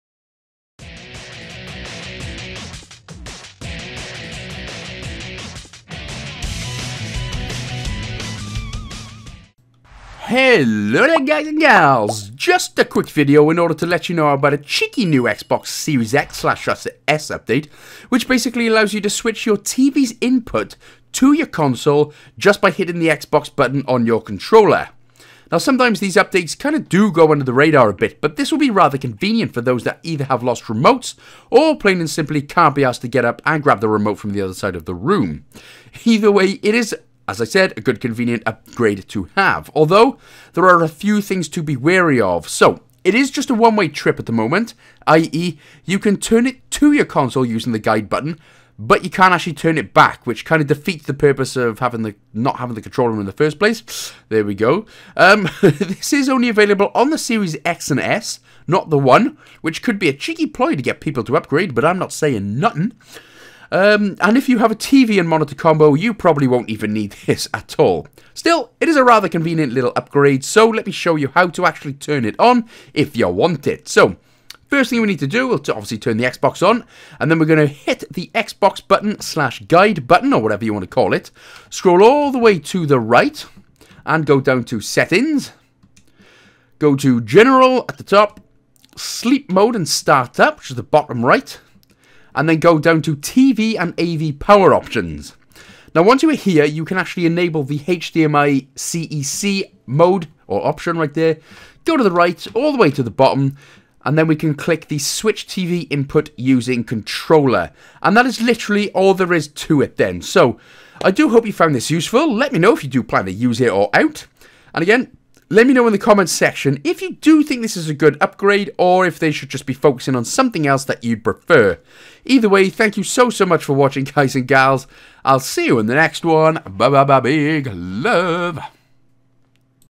New Xbox Series X/S Update - Ability to Switch Your TVs Input With JUST Your Xbox Controller!
Hey Guys! Hey Gals! Superstar Dj's, Here We Go!

Sorry, wrong one!
Anyway, Xbox have just rolled out a new update where you can switch your tvs input by simply pressing the guide/Xbox button on your remote!
I show you just how and where to turn it on incase you want the feature!

TimG84
Khost
TschanSchpange1
TonySki
Eagle 069
Lucas1987

Big Love!